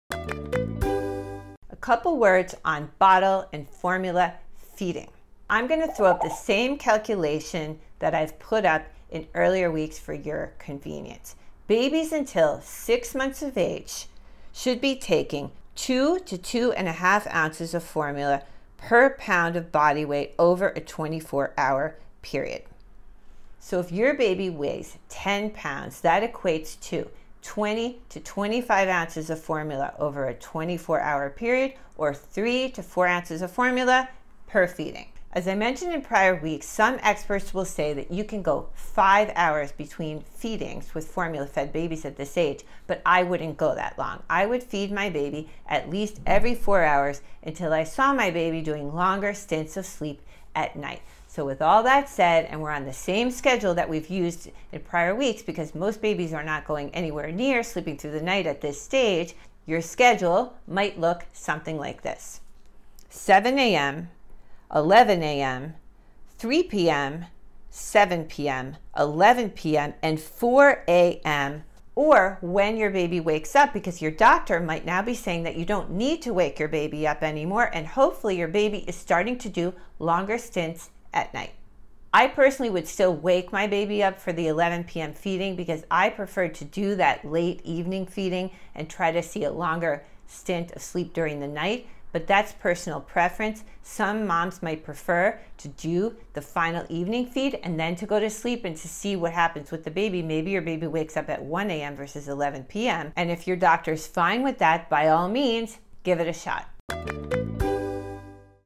5-Week-Old Baby: Bottle Feeding Schedule and Tips | Subt. ENG/ FR/ ES/ ZHO_CN | CloudMom
Find more about your 5 week old baby And watch this video for a 5 week old bottle feeding schedule and 5 week old baby feeding amount info.